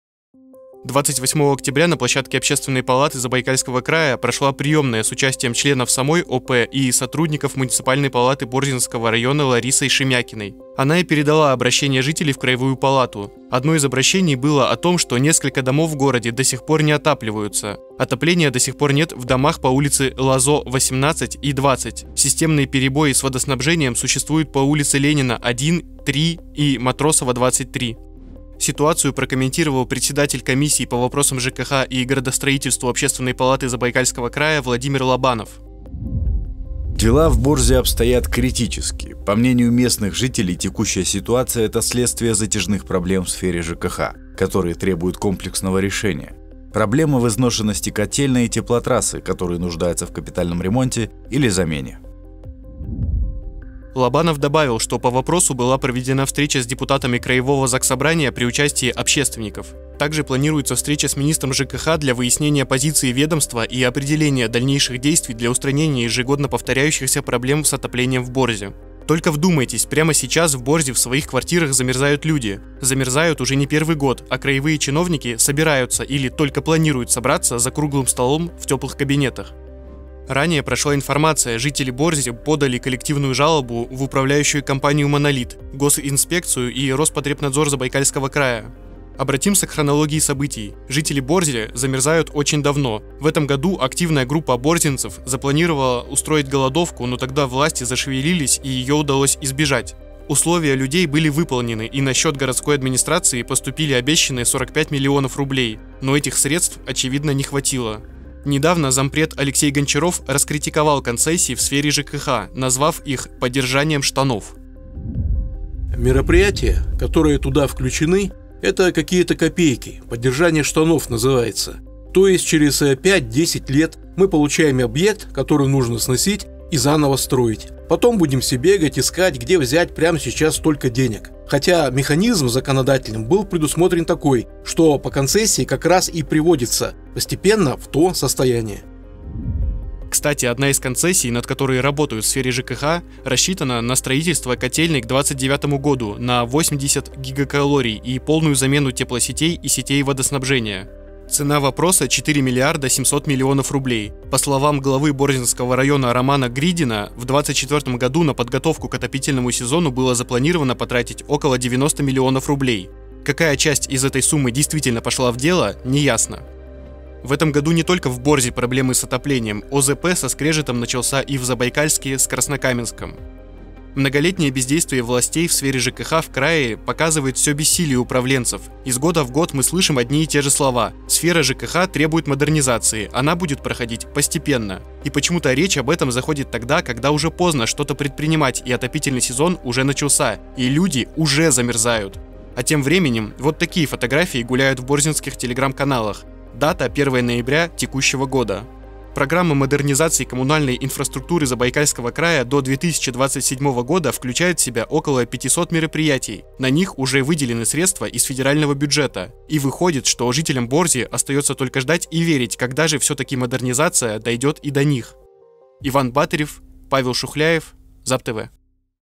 Несколько домов в Борзе до сих пор не отапливаются
Отопительный сезон в Забайкальском крае, кажется, никогда не проходит гладко. 24 год тоже не стал исключением. Жители Борзи утверждают -  в их домах до сих пор нет тепла. Подробнее в сюжете Ивана Батырева